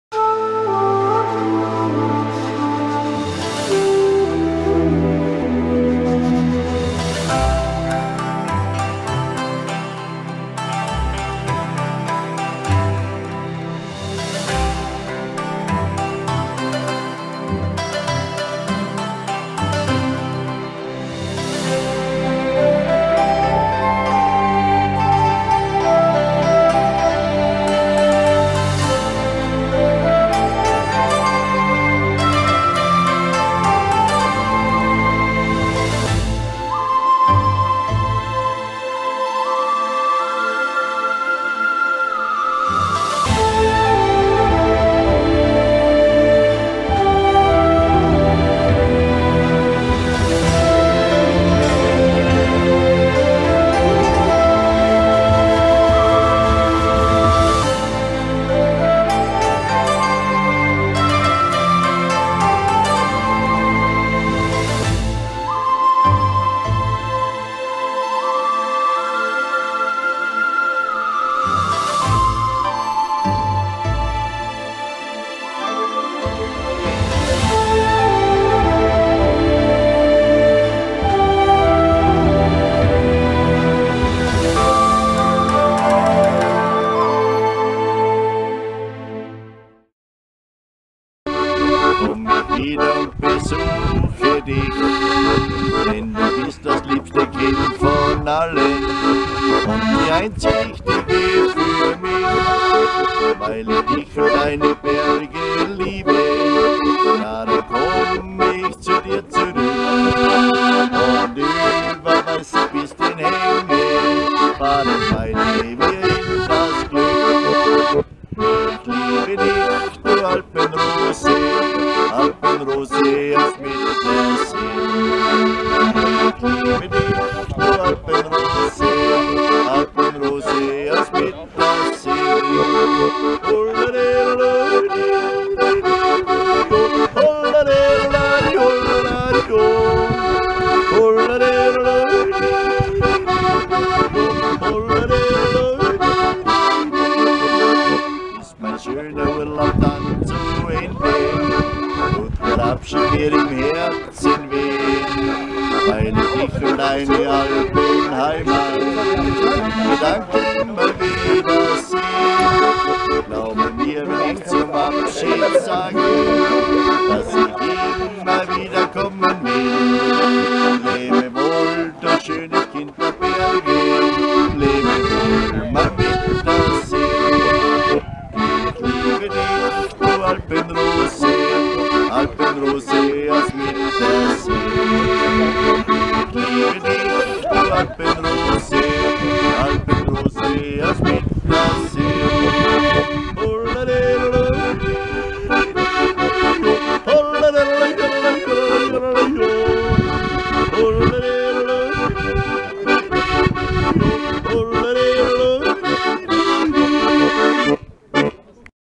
AUF DER TEICHALM
Ein kurzer Ausflug auf die Teichalm bei schönen Herbstwetter